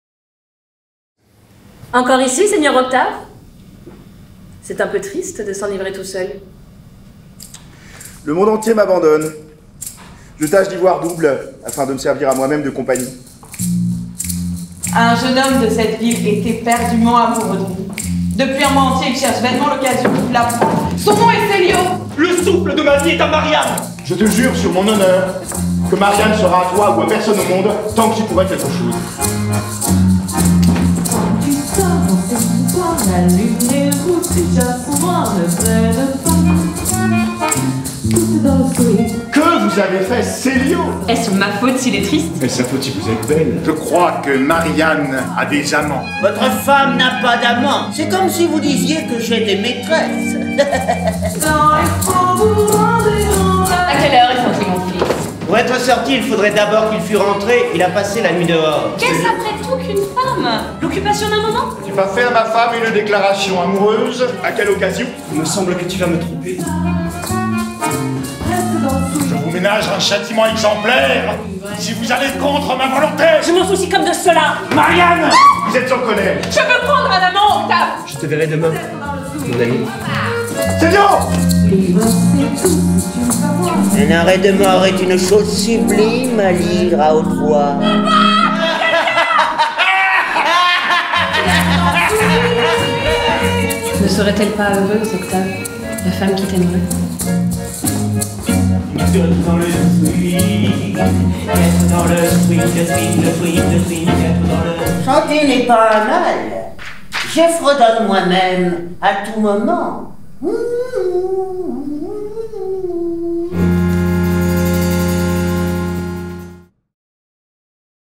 Caprices de Marianne Bande Annonce
Les Caprices de Marianne d'Alfred de Musset  A2R Cie - Antre de Rêves  Mise en scène & lumière  / Roch-Antoine Albaladéjo  Scénographie  / Roch-Antoine Albaladéjo, J.P. Delettre  Costumes / Jérôme Ragon  Création Musicales & Arrangements / Laurent Labruyère, Aliénor Gueniffet & A2R Compagnie Régie générale / Rodrigue Louisar  Dramaturgie & Coach Vocale / Marie Ballot  Avec  Mathilde Bourbin, Vincent Desprat, Valérie Français, Aliénor Gueniffet, Laurent Labruyère, Mélanie Le Duc, Jacques Poix-Terrier, Jérôme Ragon  SOUTIENS :  ADAMI / Région Bourgogne-Franche-Comté / Département de l’YonneThéâtre de Sens / Yonne en Scène / Charny-Orée-de-PuisayeLa Grange aux dîmes / ARCAL